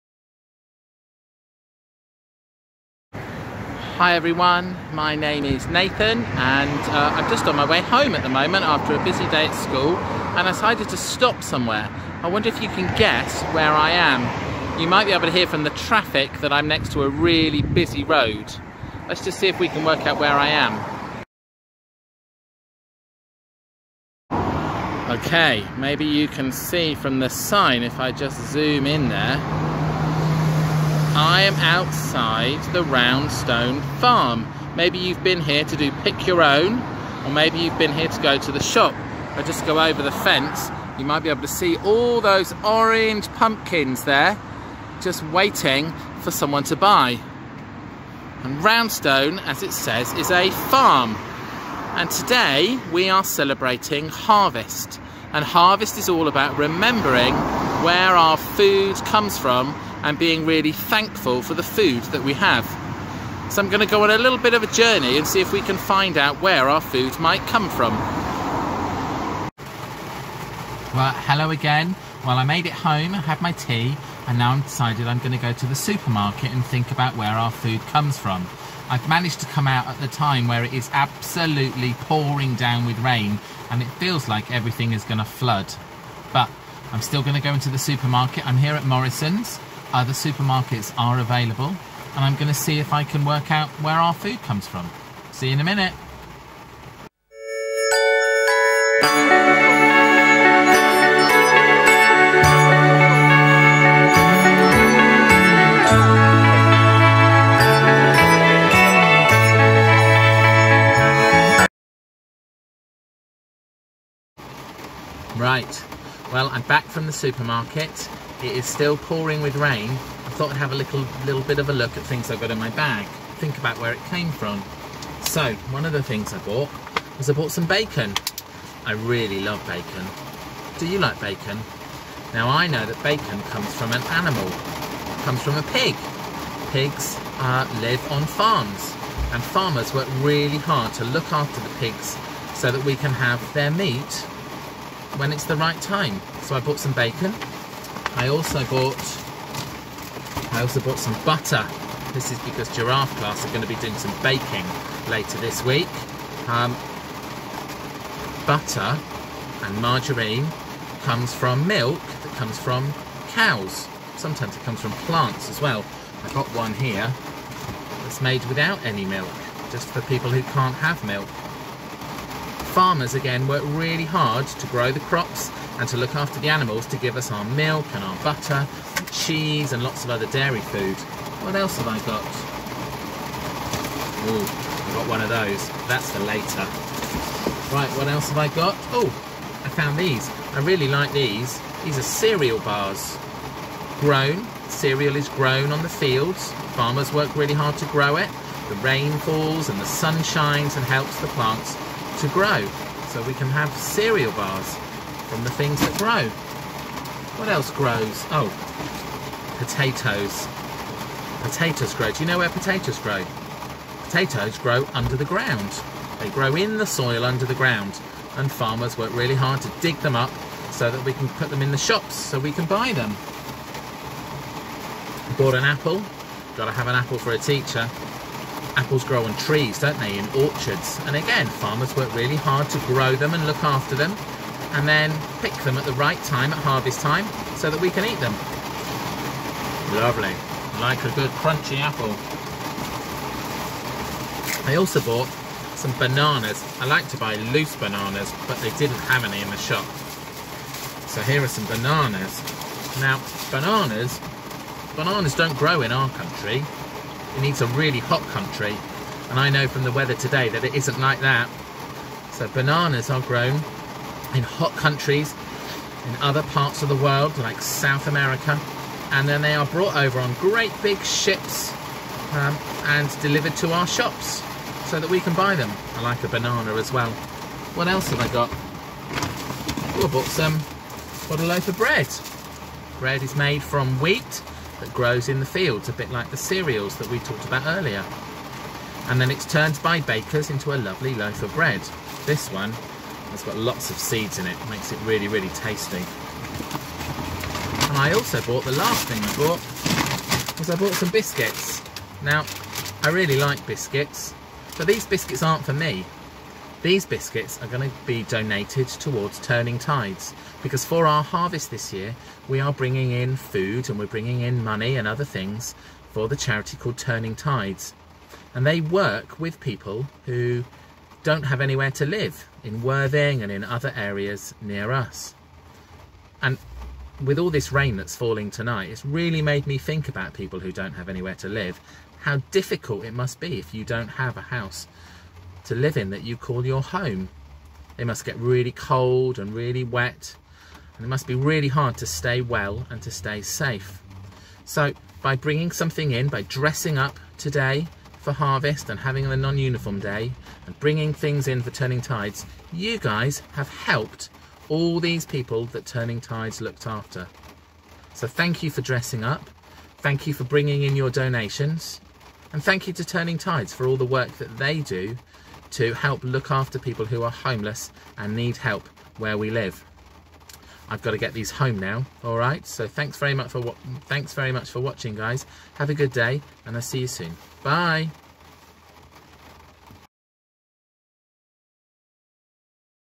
Harvest 2021
For thinkingthursday we are thinking about Harvest today. Where does your food come from? Who grows it? Go on a little journey to find out.